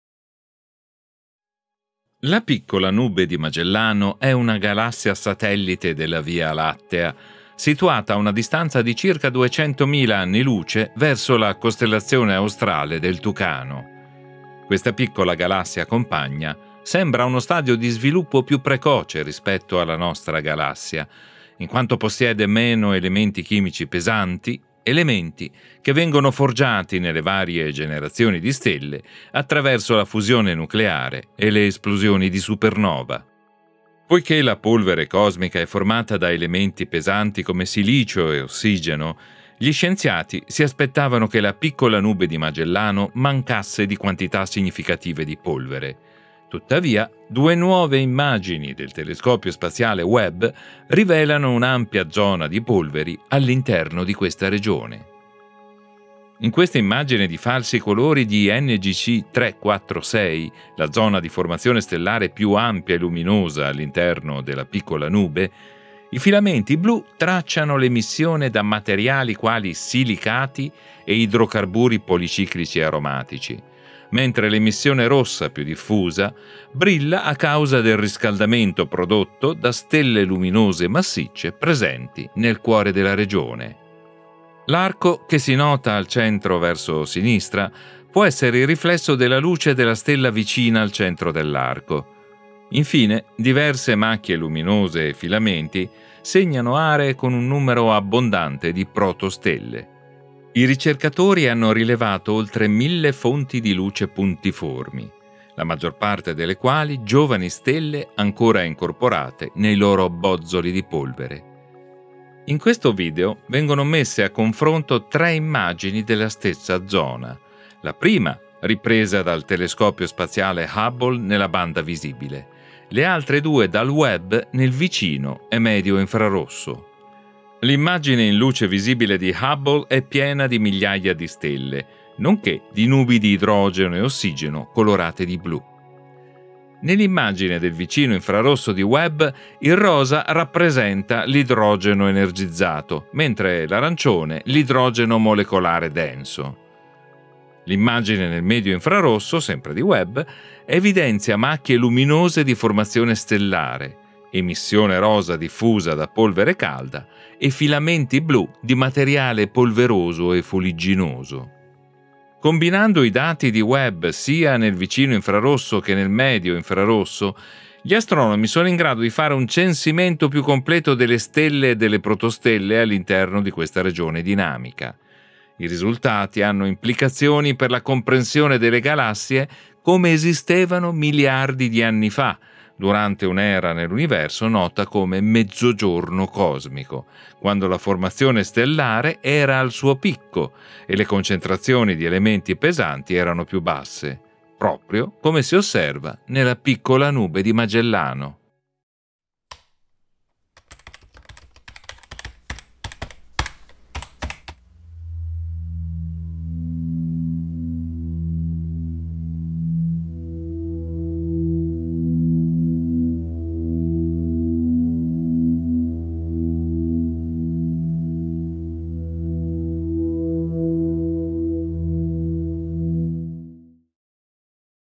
La Piccola Nube di Magellano ha più polvere del previsto
Servizio di Stefano Parisini
Crediti video: ESA/Webb, NASA, ESA, CSA, N. Habel (JPL), P. Kavanagh (Maynooth University), A. James (STScI), N. Bartmann (ESA/Webb), M. Zamani (ESA/Webb)

La Piccola Nube di Magellano è una galassia satellite della Via Lattea, situata a una distanza di circa 200mila anni luce verso la costellazione australe del Tucano. Questa piccola galassia compagna sembra a uno stadio di sviluppo più precoce rispetto alla nostra galassia, in quanto possiede meno elementi chimici pesanti, elementi che vengono forgiati nelle varie generazioni di stelle attraverso la fusione nucleare e le esplosioni di supernova.

Poiché la polvere cosmica è formata da elementi pesanti come silicio e ossigeno, gli scienziati si aspettavano che la Piccola Nube di Magellano mancasse di quantità significative di polvere. Tuttavia due nuova immagine del telescopio spaziale Webb rivelano un'ampia zona di polveri all'interno di questa regione.

In questa immagine in falsi colori di NGC 346, la zona di formazione stellare più ampia e luminosa all’interno della Piccola Nube, i filamenti blu tracciano l'emissione da materiali quali silicati e idrocarburi policiclici aromatici, mentre l'emissione rossa più diffusa brilla a causa del riscaldamento prodotto da stelle luminose e massicce presenti nel cuore della regione.

L’arco, che si nota al centro verso sinistra, può essere il riflesso della luce della stella vicina al centro dell'arco. Infine, diverse macchie luminose e filamenti segnano aree con un numero abbondante di protostelle. I ricercatori hanno rilevato oltre mille fonti di luce puntiformi, la maggior parte delle quali giovani stelle ancora incorporate nei loro bozzoli di polvere.

In questo video vengono messe a confronto tre immagini della stessa zona, la prima ripresa dal telescopio spaziale Hubble nella banda visibile, le altre due dal Webb nel vicino e medio infrarosso.

L'immagine in luce visibile di Hubble è piena di migliaia di stelle, nonché di nubi di idrogeno e ossigeno colorate di blu. Nell'immagine del vicino infrarosso di Webb, il rosa rappresenta l'idrogeno energizzato, mentre l'arancione rappresenta l'idrogeno molecolare denso. L'immagine nel medio infrarosso, sempre di Webb, evidenzia macchie luminose di formazione stellare, emissione rosa diffusa da polvere calda e filamenti blu di materiale polveroso e fuligginoso.

Combinando i dati di Webb sia nel vicino infrarosso che nel medio infrarosso, gli astronomi sono in grado di fare un censimento più completo delle stelle e delle protostelle all'interno di questa regione dinamica. I risultati hanno implicazioni per la comprensione delle galassie come esistevano miliardi di anni fa, durante un'era nell'universo nota come "mezzogiorno cosmico", quando la formazione stellare era al suo picco e le concentrazioni di elementi pesanti erano più basse, proprio come si osserva nella Piccola Nube di Magellano.